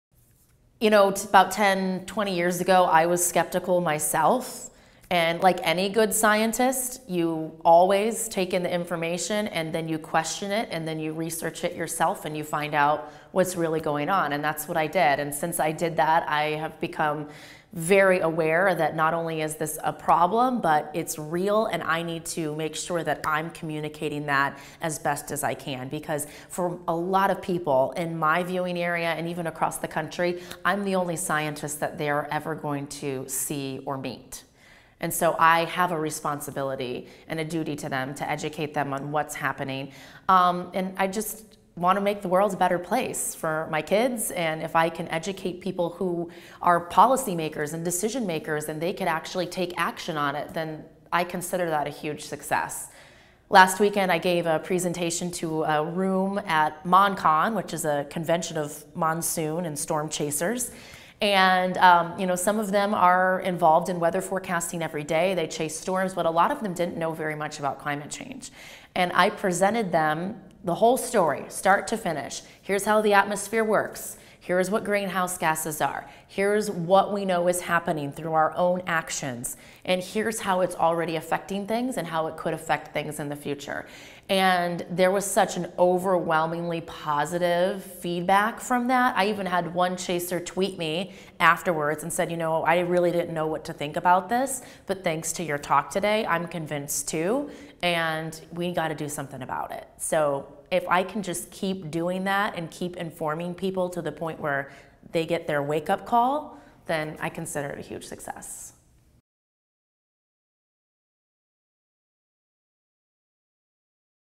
TV Meteorologist Amber Sullins: No Longer Skeptical of Climate Science
Amber Sullins of Phoenix ABC 15 on the importance of heeding the science of climate change.